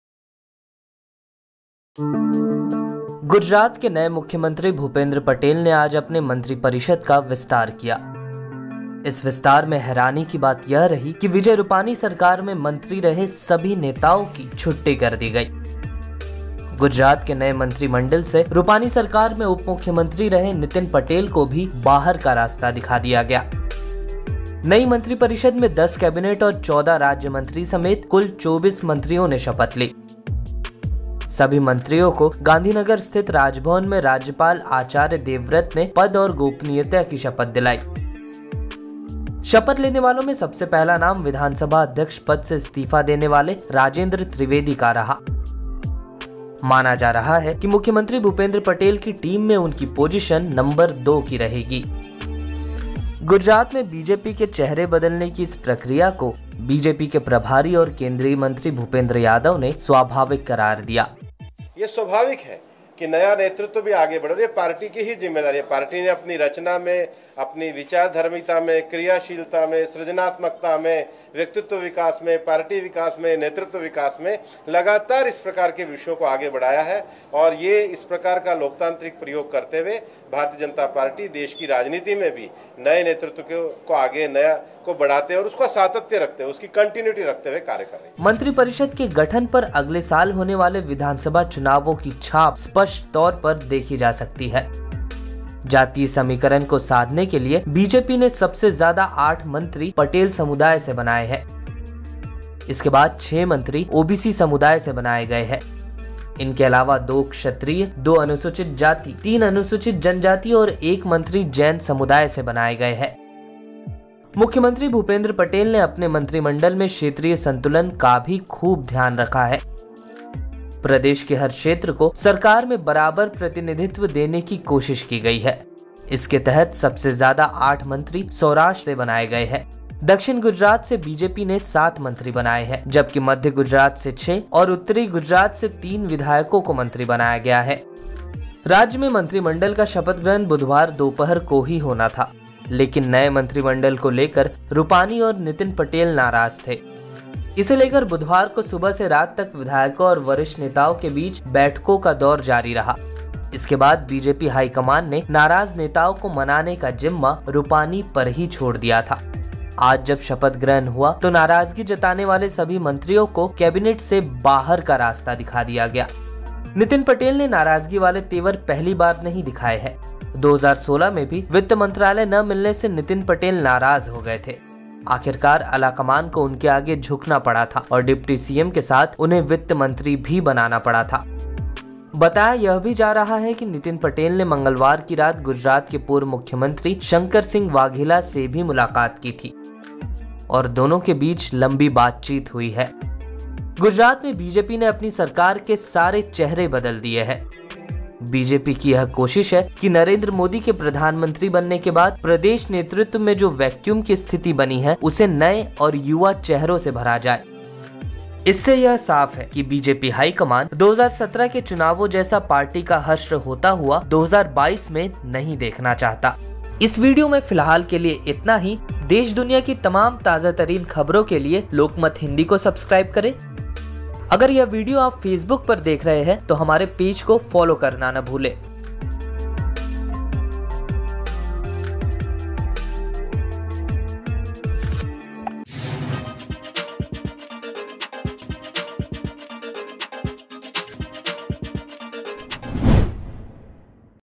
Gujarat New Cabinet । BJP का नया Gujarat Model, CM के साथ बदल दी पूरी सरकार,Nitin Patel की भी छुट्टी
गुजरात (Gujarat) के नए मुख्यमंत्री Bhupendra Patel ने आज अपने मंत्रिपरिषद का विस्तार किया. इस विस्तार में हैरानी की बात यह रही कि Vijay Rupani सरकार में मंत्री रहे सभी नेताओं की छुट्टी कर दी गई. गुजरात के नए मंत्रिमंडल से Rupani सरकार में उप-मुख्यमंत्री रहे Nitin Patel को भी बाहर का रास्ता दिखा दिया गया.